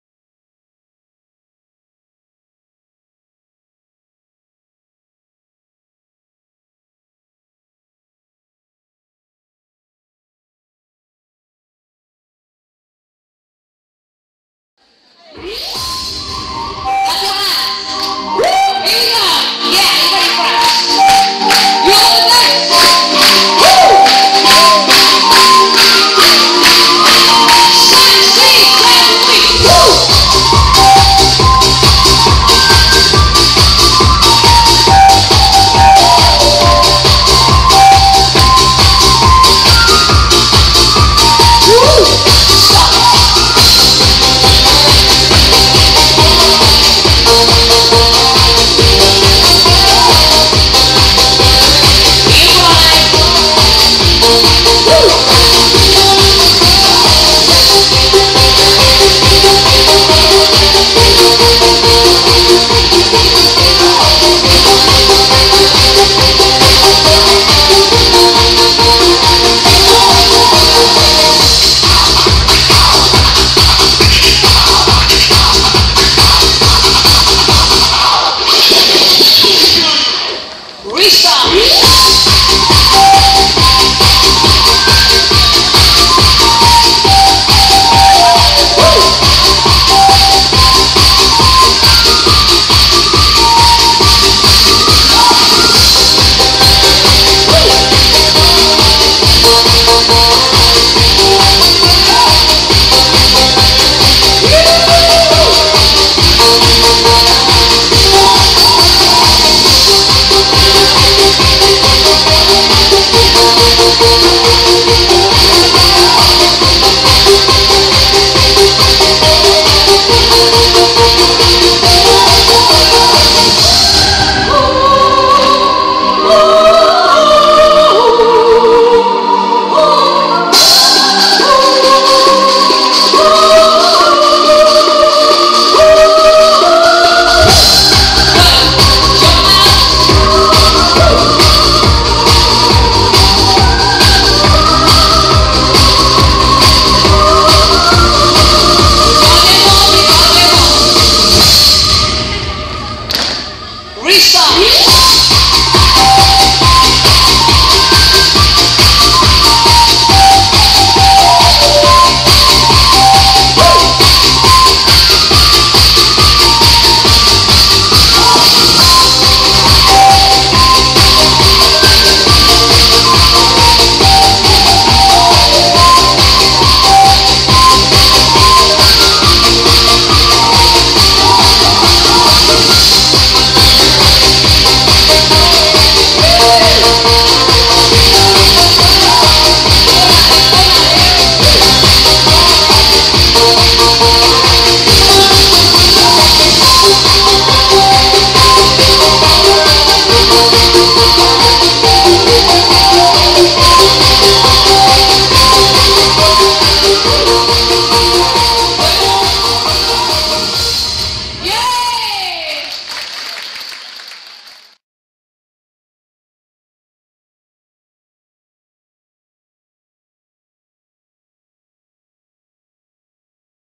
THE FLUTE Line Dance
Choreographed by: Maggie Gallagher (May 2011)
Descriptions: 64 count - 3 wall - Intermediate level line dance
Perfomed by Gabrielle and the dancers at The Median, St Quentin Fallavier, France, 10/28/2011